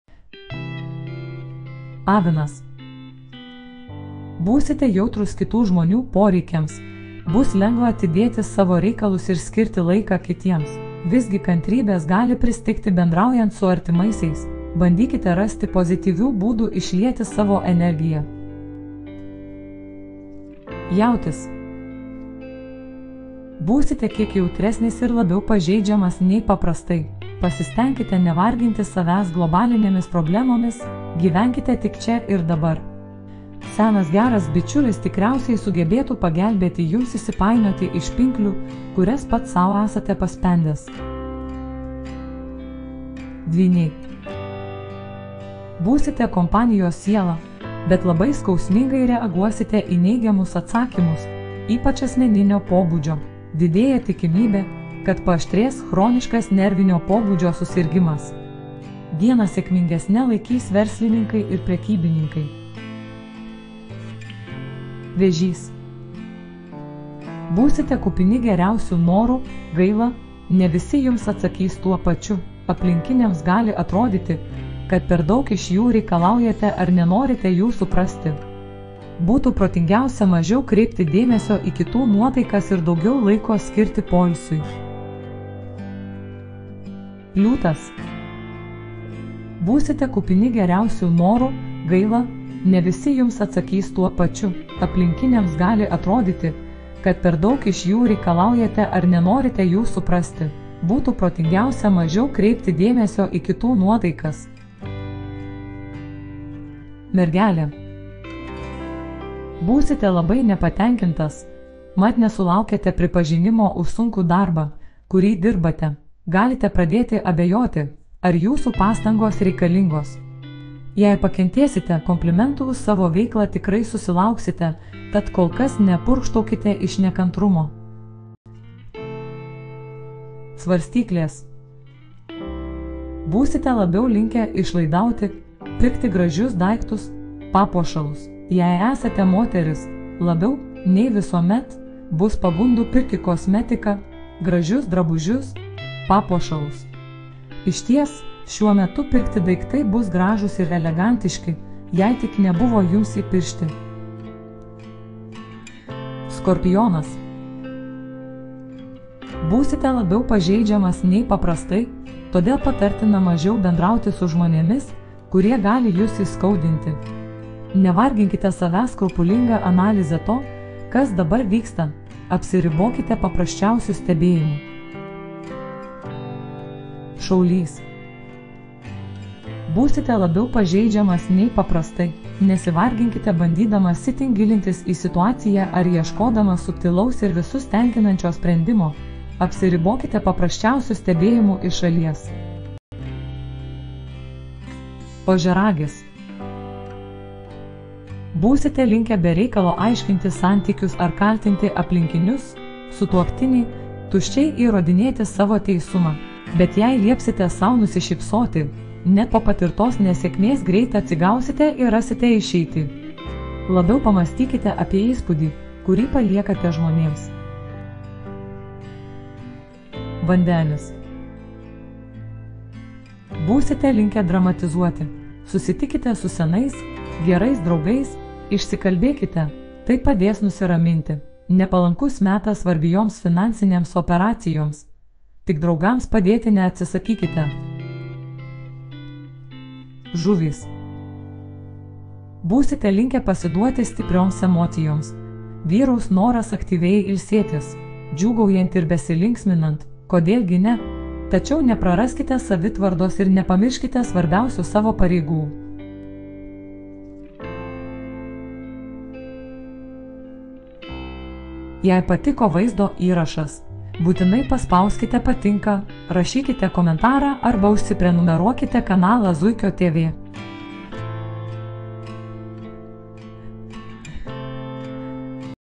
Kovo 30 d. horoskopas
Kovo 30 d. horoskopas dvylika Zodiako ženklų.

00:00:00 Avinas
00:00:18 Jautis
00:00:41 Dvyniai
00:01:02 Vėžys
00:01:25 Liūtas
00:01:44 Mergelė
00:02:08 Svarstyklės
00:02:34 Skorpionas
00:02:54 Šaulys
00:03:12 Ožiaragis
00:03:37 Vandenis
00:03:57 Žuvys